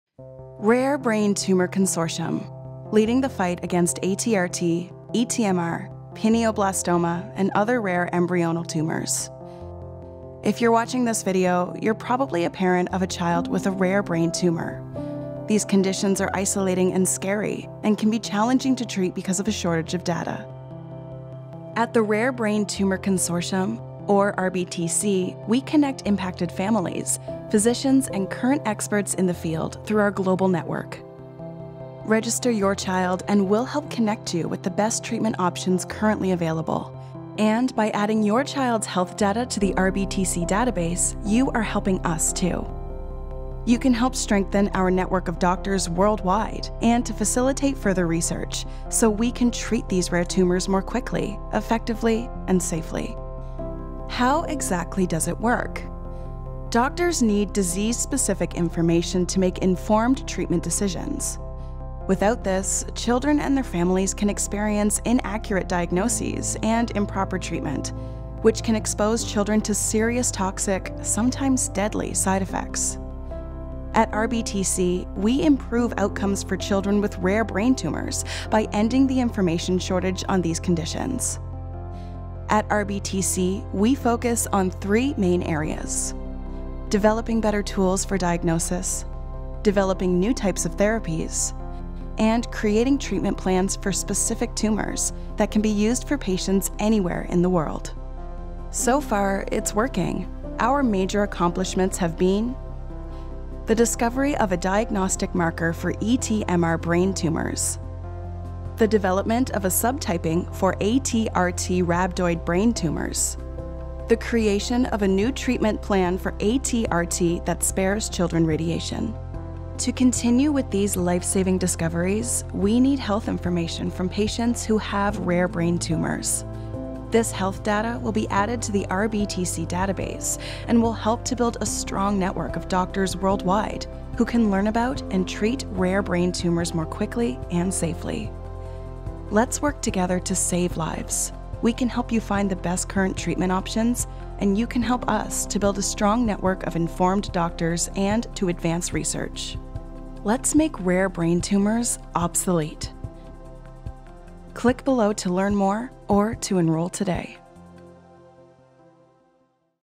Rare Brain Tumor Consortium: Working to find cures for ATRTs, ETMRs & other rare brain tumors
The Rare Brain Tumor Consortium is an international network of physicians and scientists dedicated to making rare brain tumors in children a curable disease. Join us!